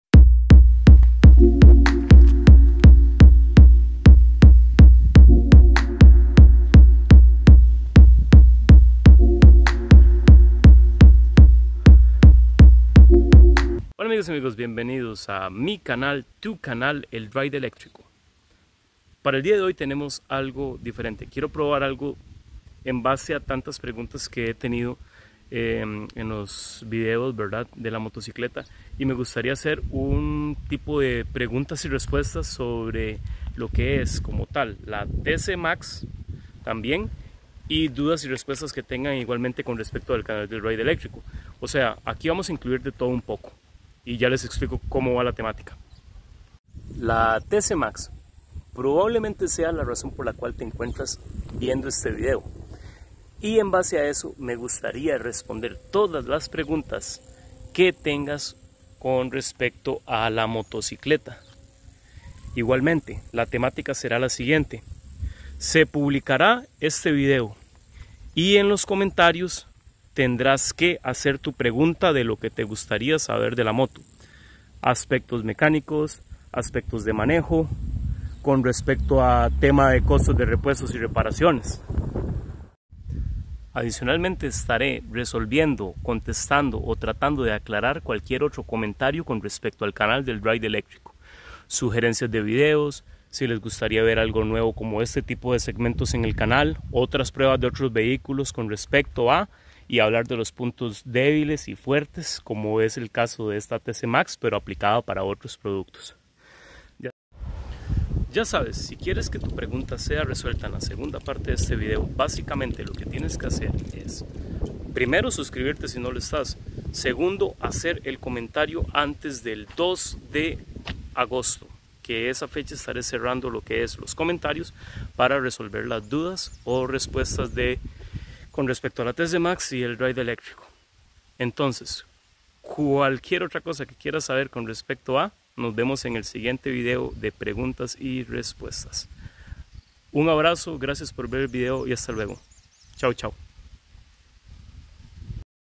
Preguntas y Respuesta Super Soco TC Max Parte 1
El video consiste en que te animes en hacer tus consultas y comentarios sobre TC Max y el canal El Ride Eléctrico. Realiza tu comentario y si le doy corazón al mismo las estaré resolviendo en la segunda parte, posterior al primero de agosto.
Gracias por tanto!

PURA VIDA